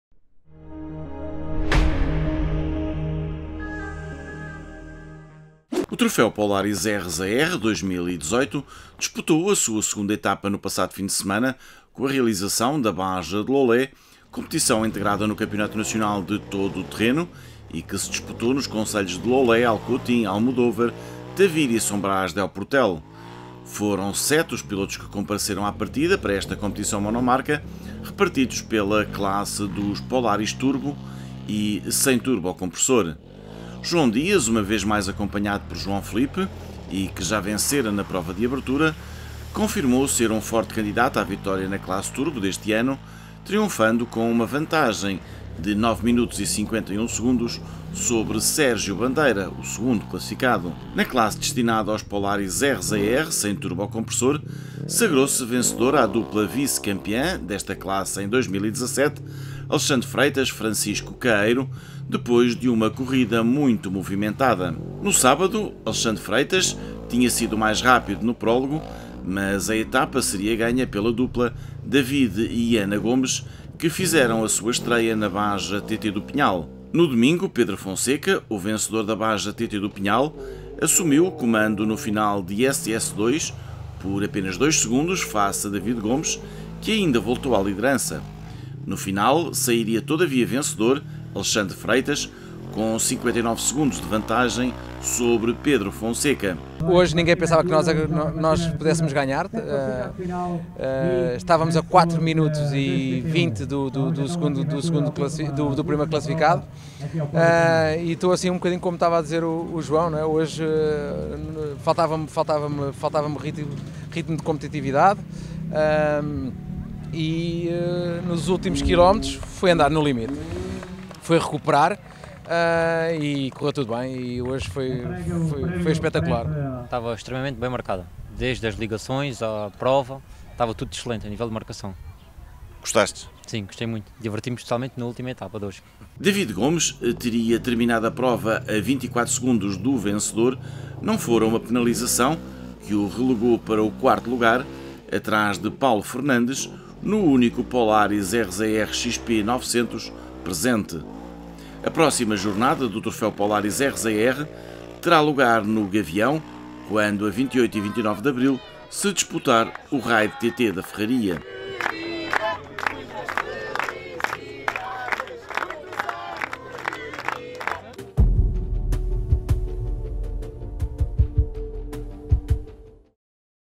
Troféu Polaris: João Dias e Alexandre Freitas vencem Baja de Loulé
O Troféu Polaris RZR 2018 disputou a sua segunda etapa no passado fim de semana com a realização da Baja de Loulé, competição integrada no Campeonato Nacional de Todo-o-Terreno 2018.

Foram sete os pilotos que compareceram à partida para esta competição monomarca, repartidos pela classe dos Polaris Turbo (3) e sem Turbocompressor (4).

João Dias, uma vez mais acompanhado por João Filipe e que já vencera na prova de abertura, confirmou ser um forte candidato à vitória da Classe Turbo deste ano, triunfando com uma vantagem de 9m51s sobre Sérgio Bandeira, o 2º classificado.

Na classe destinada aos Polaris RZR sem Turbocompressor sagrou-se vencedora a dupla vice-campeã desta classe em 2017, Alexandre Freitas/Francisco Caeiro depois de uma corrida muito movimentada.
No sábado Alexandre Freitas foi o mais rápido no prólogo, mas a etapa seria ganha pela dupla David e Ana Gomes que fizeram a sua estreia na Baja TT do Pinhal.

No domingo, Pedro Fonseca, o vencedor da Baja TT do Pinhal, assumiu o comando no final de SS2 por apenas 2s face a David Gomes que ainda voltou à liderança. No final sairia, todavia, vencedor Alexandre Freitas com 59s de vantagem sobre Pedro Fonseca. David Gomes teria terminado a 24s do vencedor não fora uma penalização que o relegou para o 4º lugar, atrás de Paulo Fernandes, no único Polaris RZR XP 900 presente.

A próxima jornada do Troféu Polaris RZR terá lugar no Gavião quando a 28 e 29 de abril se disputar o Raid TT da Ferraria.